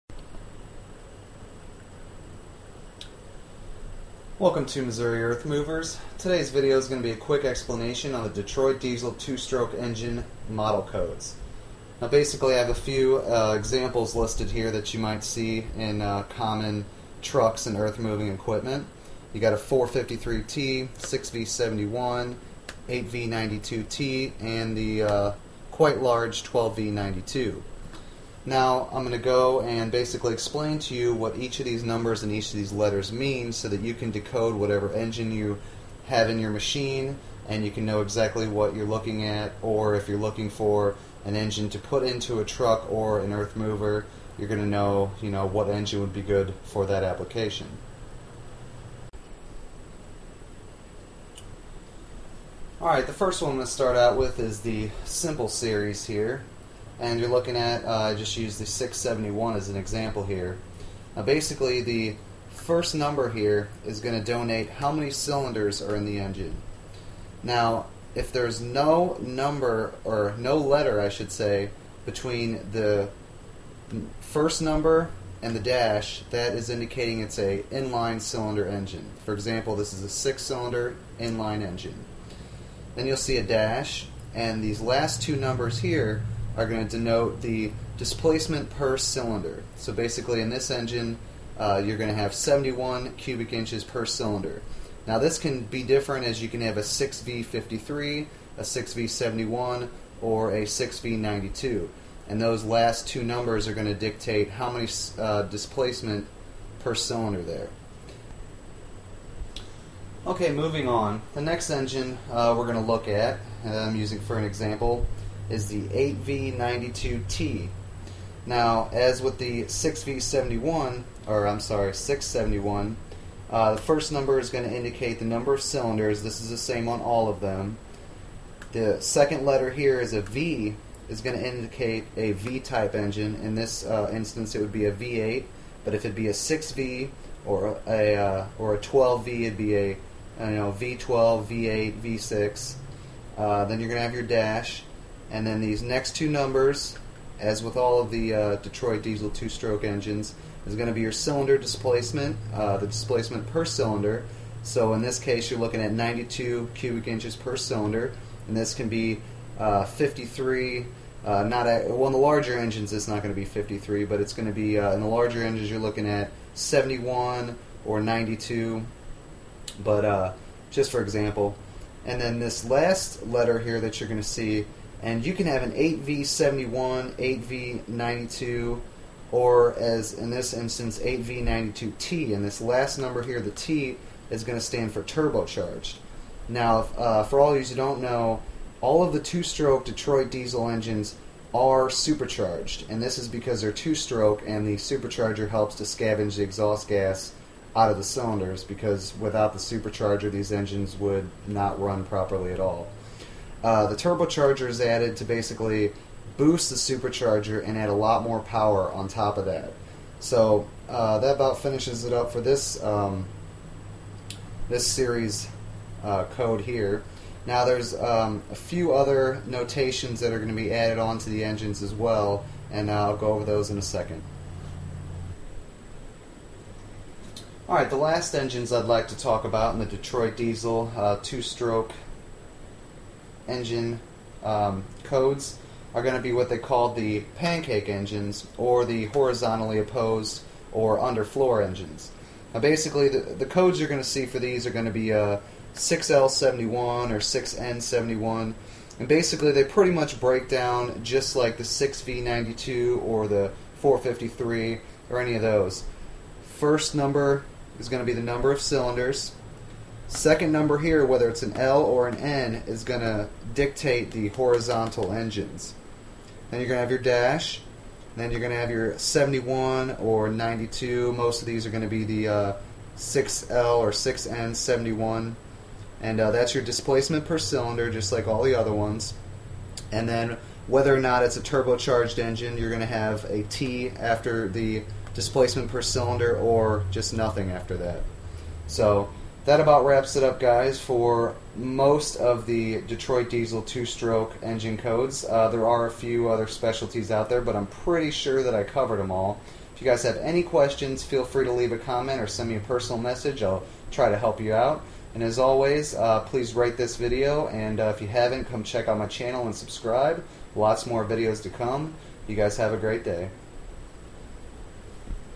Detroit Diesel 2-Stroke Engine Codes Explained
Detroit Diesel 2-Stroke Engine Model Names Explanation

Sorry for my word mix up I mentioned number instead of letter and vise versa a few times.